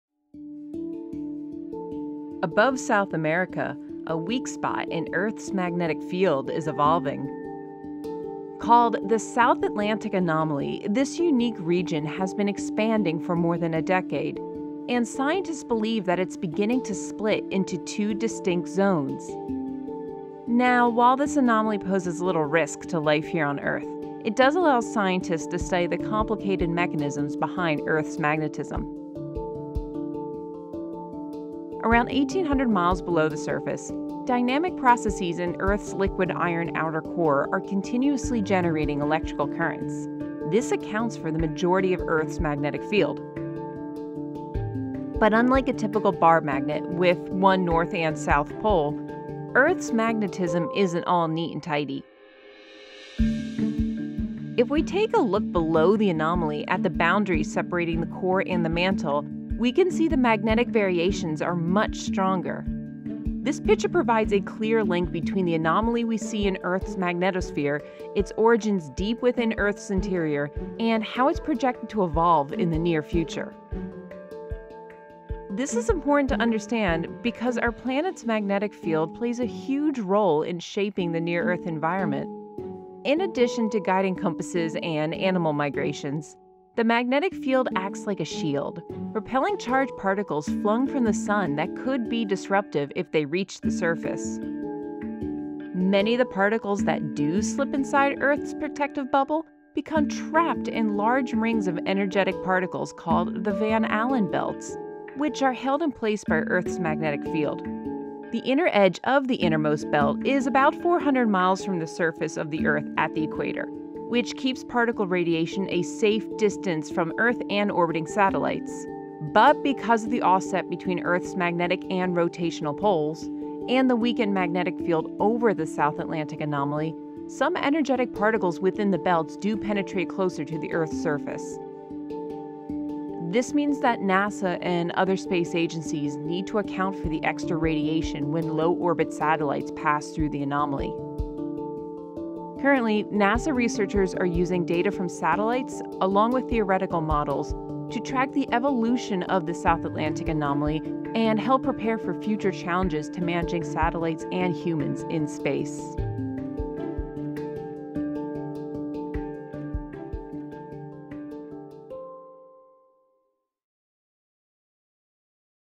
NASA Explores Earth's Magnetic "Dent"
Earth’s magnetic field acts like a protective shield around the planet, repelling and trapping charged particles from the Sun. But over South America and the southern Atlantic Ocean, an unusually weak spot in the field – called the South Atlantic Anomaly, or SAA – allows these particles to dip closer to the surface than normal.

Currently, the SAA creates no visible impacts on daily life on the surface. However, recent observations and forecasts show that the region is expanding westward and continuing to weaken in intensity. It is also splitting – recent data shows the anomaly’s valley, or region of minimum field strength, has split into two lobes, creating additional challenges for satellite missions.

The South Atlantic Anomaly is also of interest to NASA’s Earth scientists who monitor the changes in magnetic strength there, both for how such changes affect Earth's atmosphere and as an indicator of what's happening to Earth's magnetic fields, deep inside the globe.